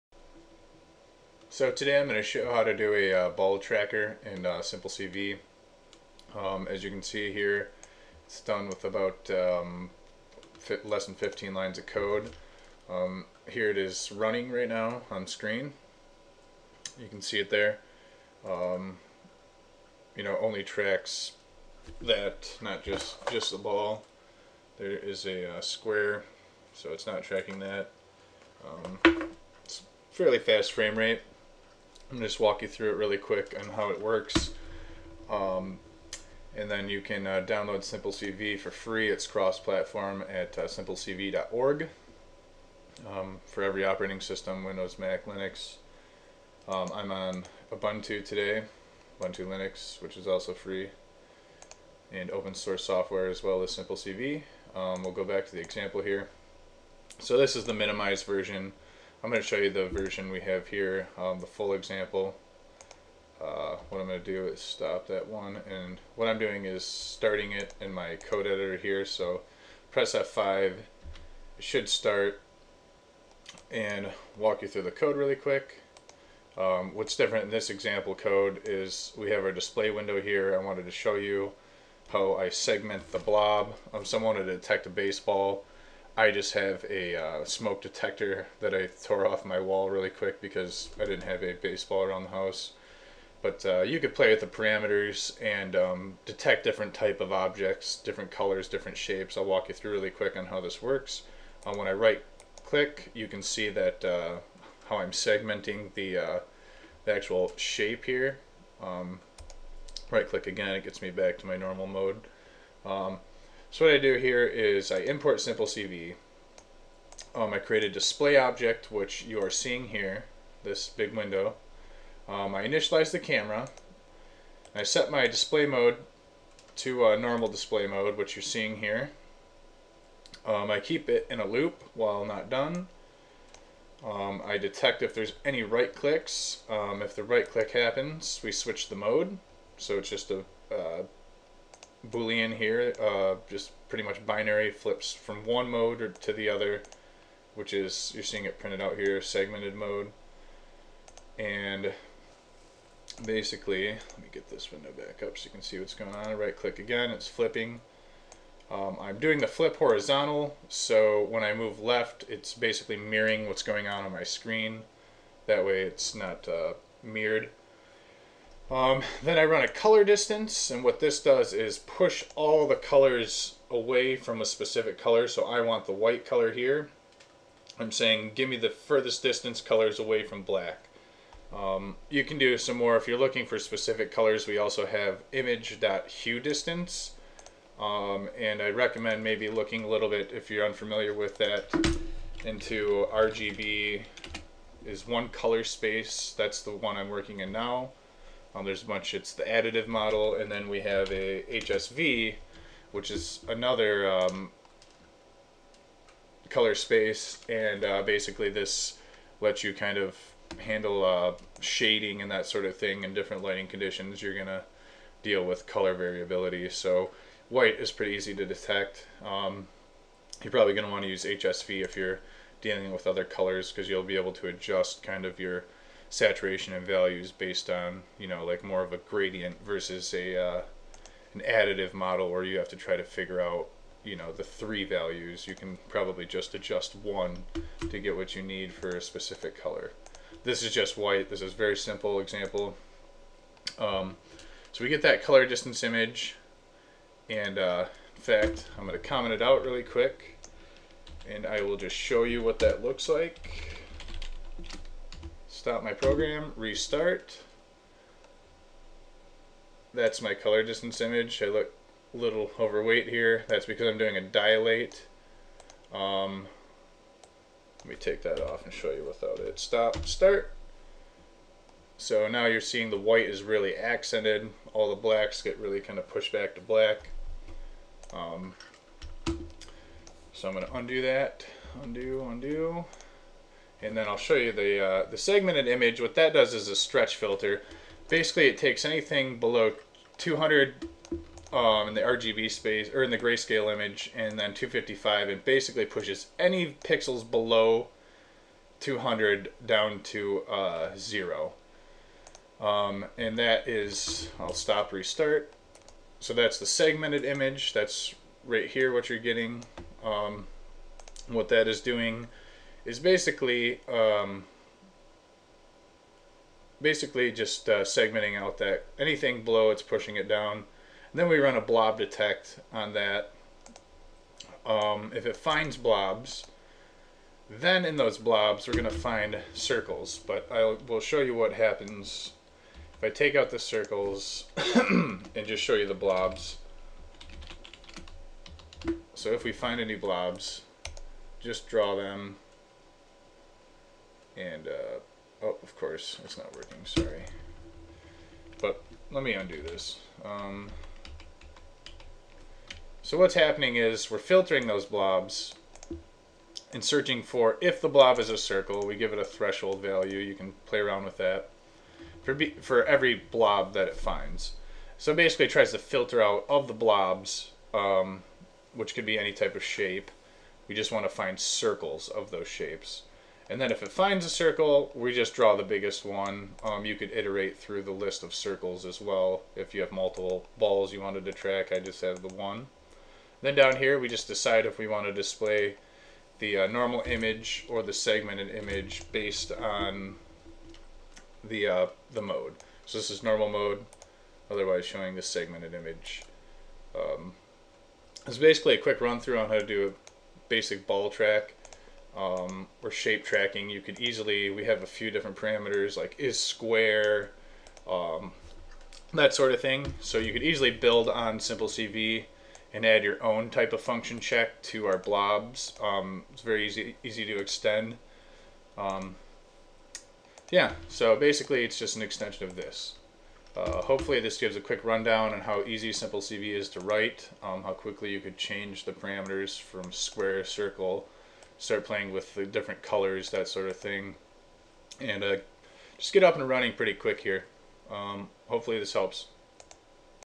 Object (White Ball) tracker - 15 lines of code - blurred face test
I am able to easily track a white ball colored object.  I found someone who was trying to track a baseball, and figured this is basically the equivalent.  I didn't have a baseball around the house so I just tore the smoke detector off my wall.